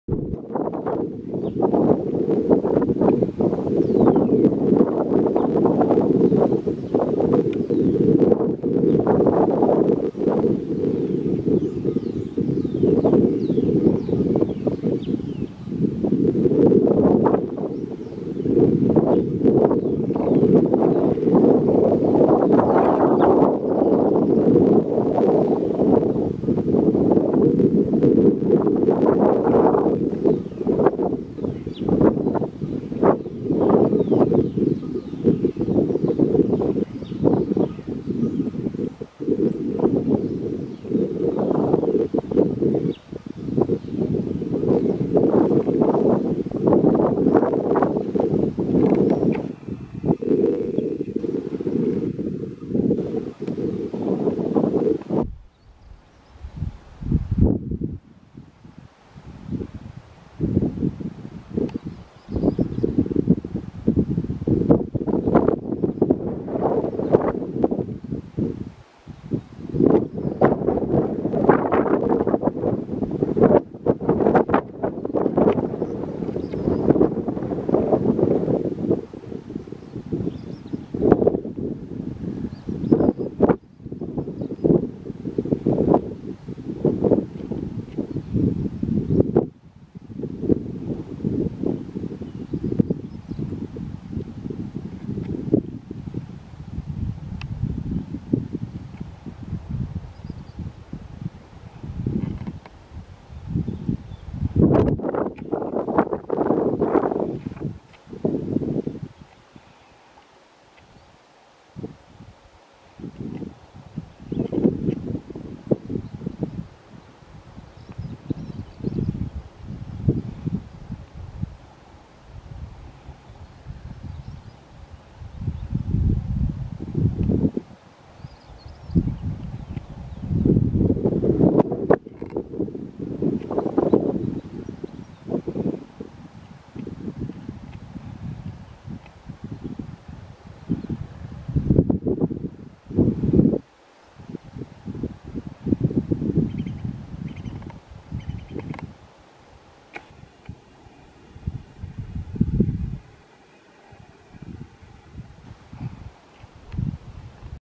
Par de perdizes-vermelhas (Alectoris rufa) na Quinta do Pisão
Duas perdizes a passear na Quinta do Pisão, Cascais.